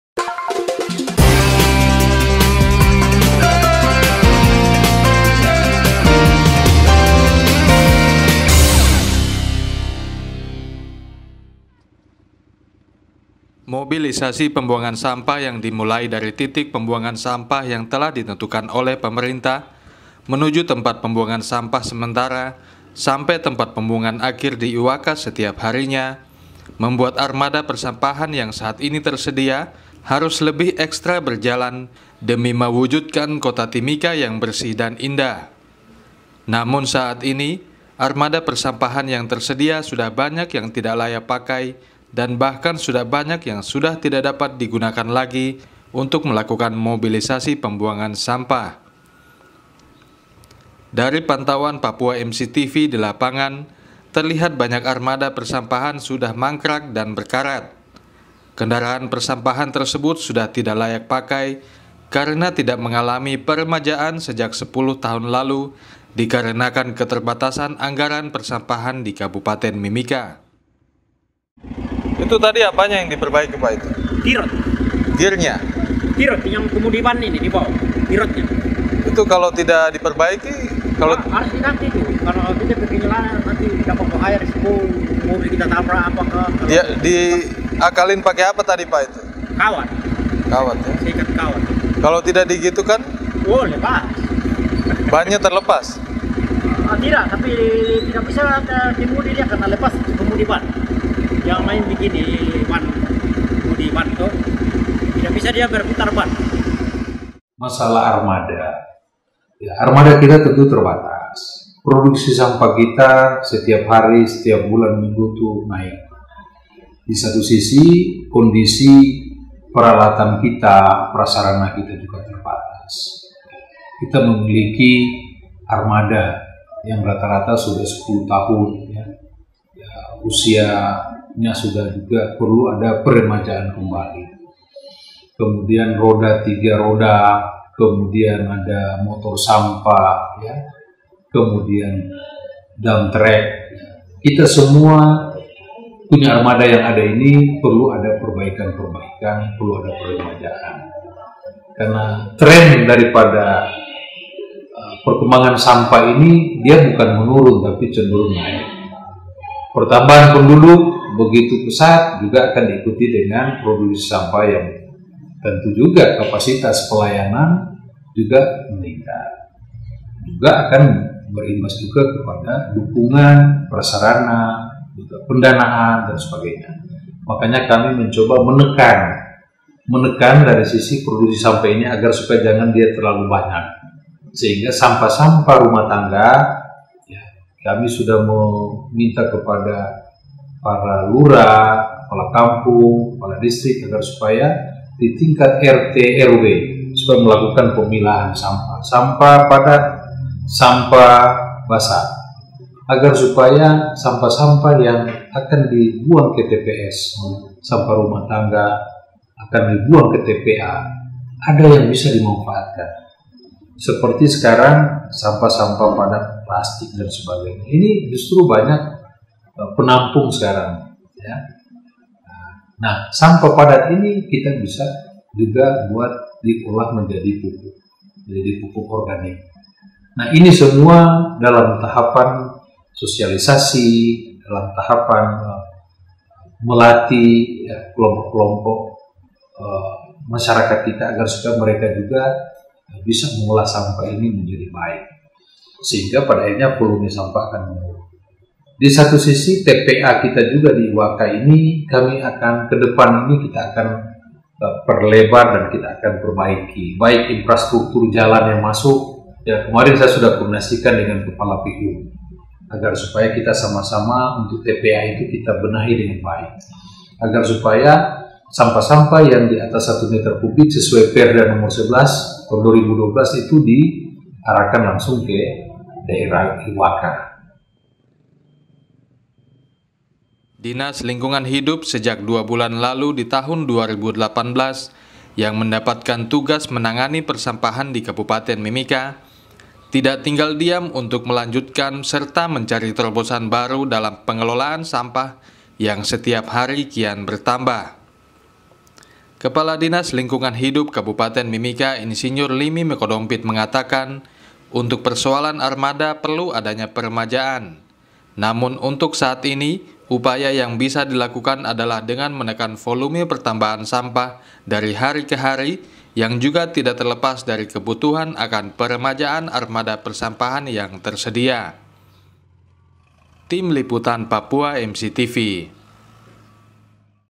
ARMADA PERSAMPAHAN PERLU PEREMAJAAN
Armada Persampahan di Kab.Mimika Kini perlu adanya peremajaan setelah sekitar 10 tahun beroperasi karena kondisinya saat ini yang sudah tidak layak jalan.